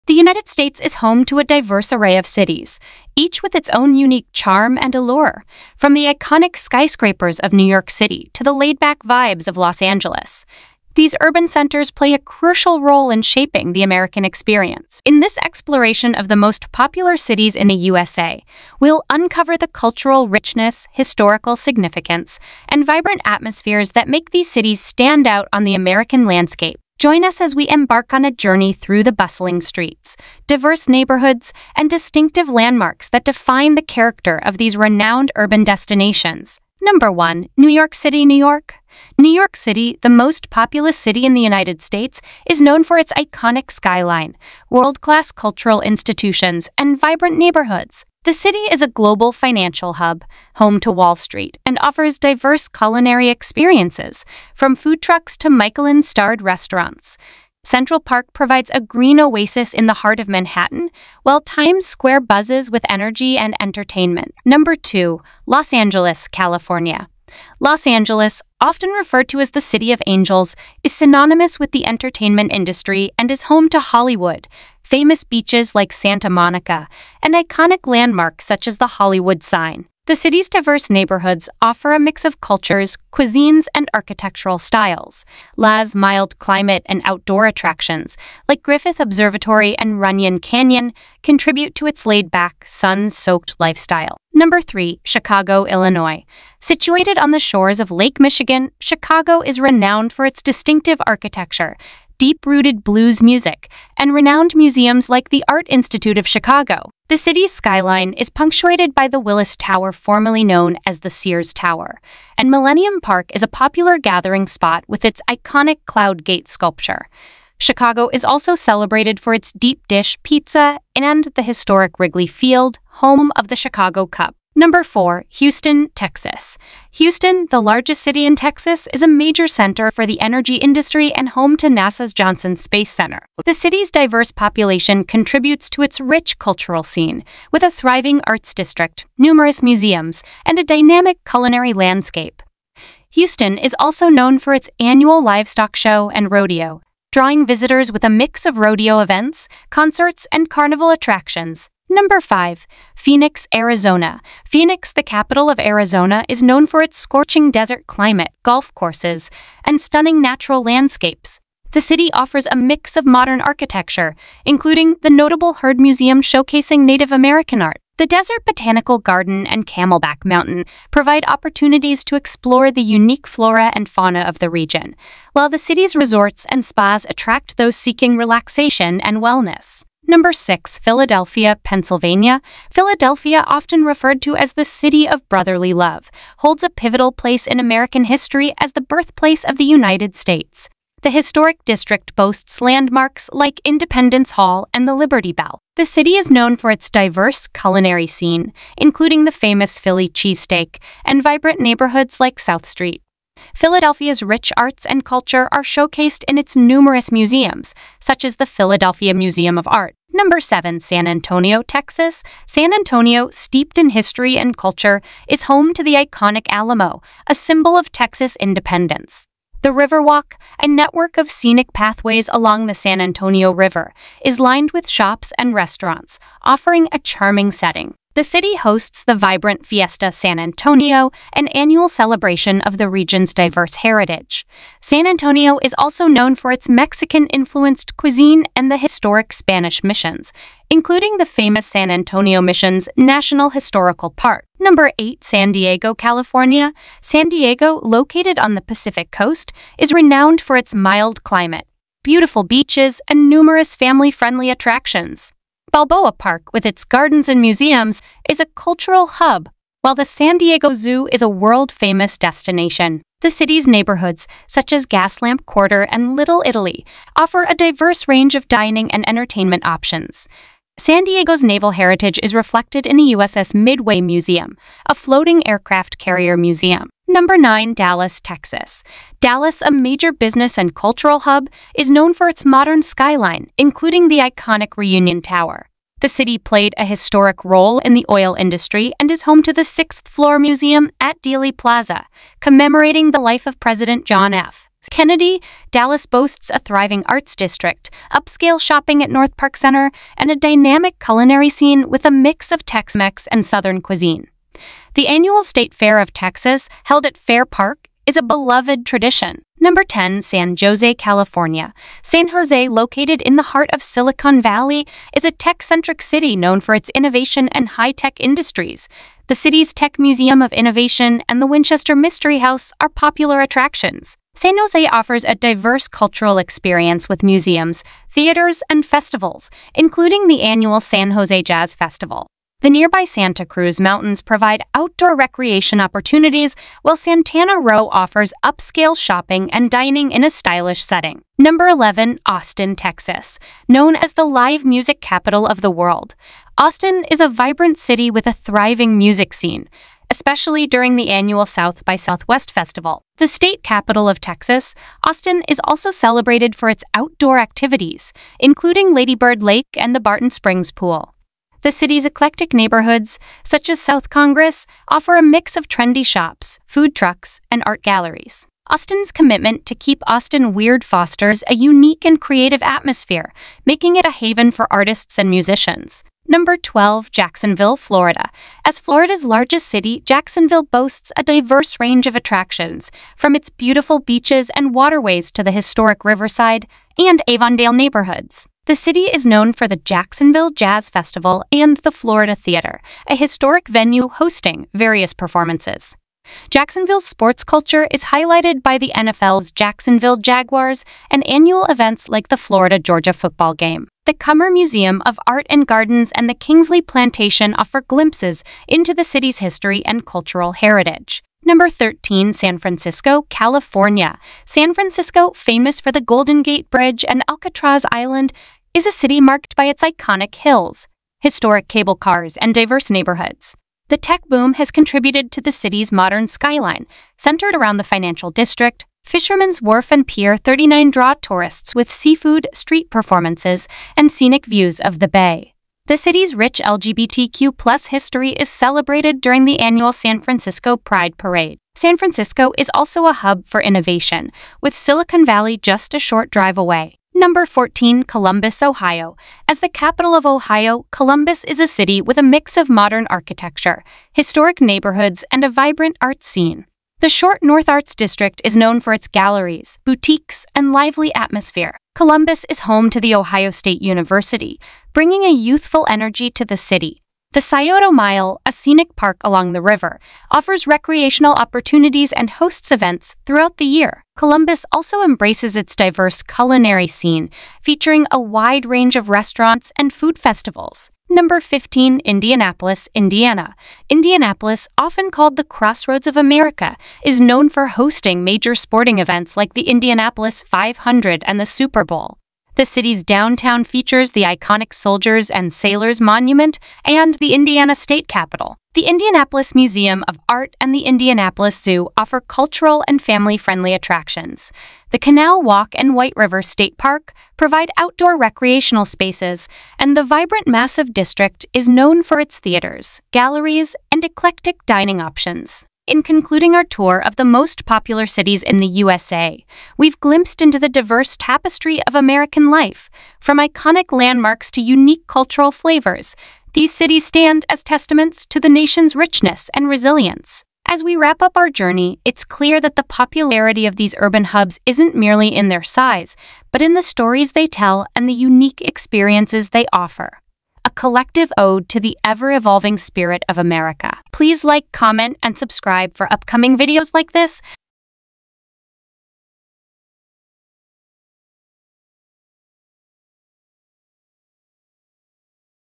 the Top 5 Must-Visit Cities in the USA! | iFTL
Welcome to a captivating journey through the heart and soul of the United States! In this video, we embark on an exploration of the most popular cities in the USA, uncovering the unique stories and vibrant cultures that make each one a standout destination.

🏙️ City Highlights:
Discover the iconic landmarks, cultural gems, and hidden treasures that define the character of America's favorite urban hubs. From the towering skyscrapers of New York City to the sun-soaked beaches of Los Angeles, we'll traverse the diverse landscapes that make these cities so beloved.

🌆 Cultural Richness:
Delve into the cultural tapestry of each city, exploring art, music, and cuisine that reflect the melting pot of influences that shape American life. Whether it's the deep blues of Chicago, the cinematic allure of Los Angeles, or the historic significance of Philadelphia, each city has a unique story to tell.

🗺️ Historic Significance:
Uncover the historical landmarks that stand as testaments to the nation's past. From Independence Hall in Philadelphia to the Alamo in San Antonio, these cities hold key chapters of American history, waiting to be explored.

🌐 Global Impact:
Explore how these cities contribute to the global stage. Whether it's the financial powerhouse of New York City, the tech innovation of San Francisco, or the cultural influence of New Orleans, these urban centers shape not only the American experience but also have a lasting impact worldwide.

🤔 Why These Cities Matter:
Join us in understanding why these cities matter, not just for their size and popularity, but for the unique experiences they offer. Each city is a vibrant character in the grand narrative of the USA, contributing to the nation's richness and diversity.

If you're ready to embark on this virtual tour of the most popular cities in the USA, hit that like button, leave a comment sharing your favorite city, and subscribe for more exciting explorations of fascinating destinations. Let's celebrate the cultural mosaic that is the United States! 🇺🇸✨